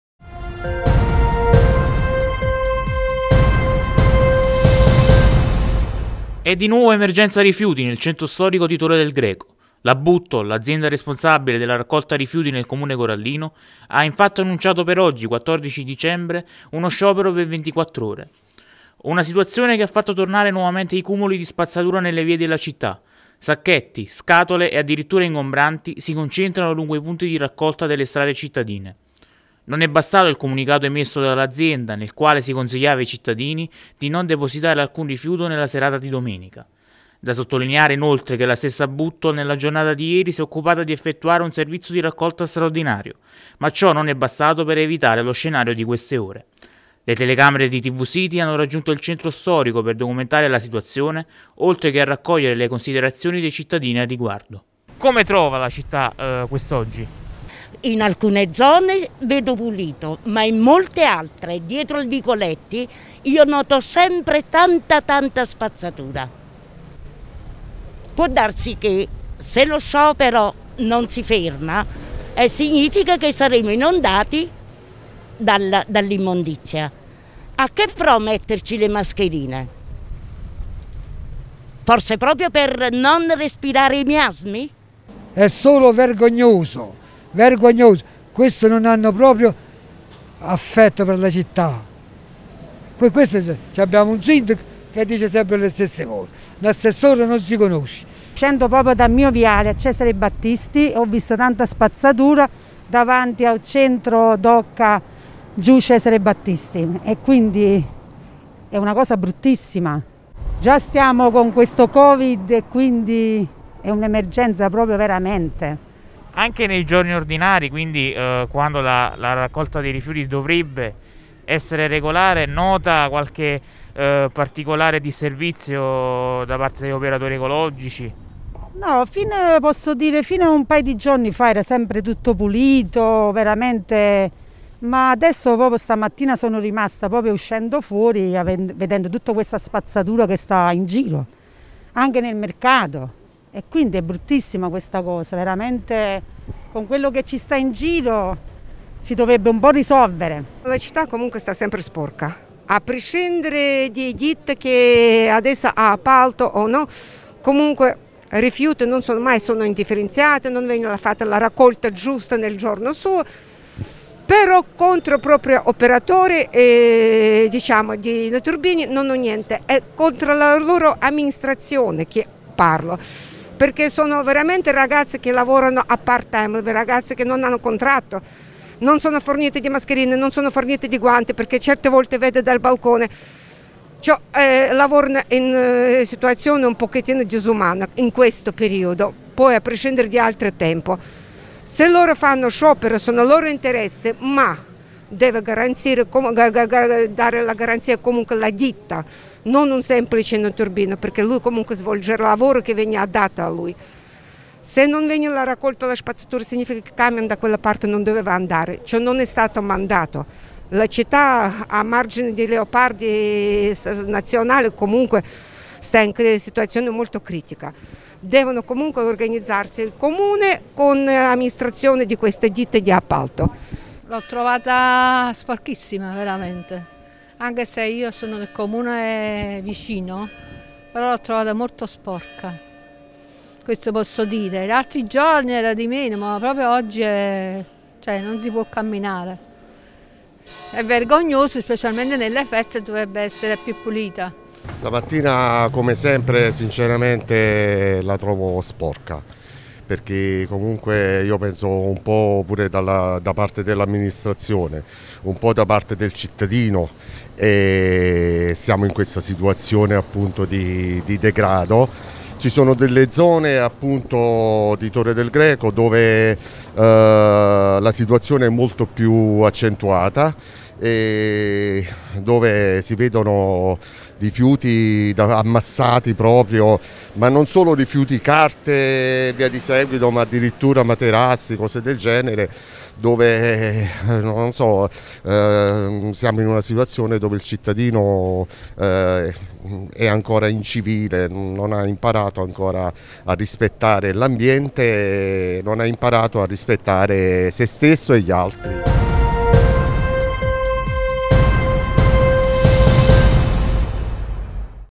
Buttol in sciopero, Torre del Greco invasa dai rifiuti - VIDEO
E’ di nuovo emergenza rifiuti nel centro storico di Torre del Greco. La Buttol, l’azienda responsabile della raccolta rifiuti nel comune corallino, ha infatti annunciato per oggi, 14 dicembre, uno sciopero per 24 ore.

Una situazione che ha fatto tornare nuovamente i cumuli di spazzatura nelle vie della città. Sacchetti, scatole e addirittura ingombranti si concentrano lungo i punti di raccolta delle strade cittadine.

Non è bastato il comunicato emesso dall’azienda, nel quale si consigliava ai cittadini di non depositare alcun rifiuto nella serata di domenica.

Da sottolineare inoltre che la stessa Buttol nella giornata di ieri si è occupata di effettuare un servizio di raccolta straordinario, ma ciò non è bastato per evitare lo scenario di queste ore.

Le telecamere di TVCITY hanno raggiunto il centro storico per documentare la situazione, oltre che a raccogliere le considerazioni dei cittadini a riguardo.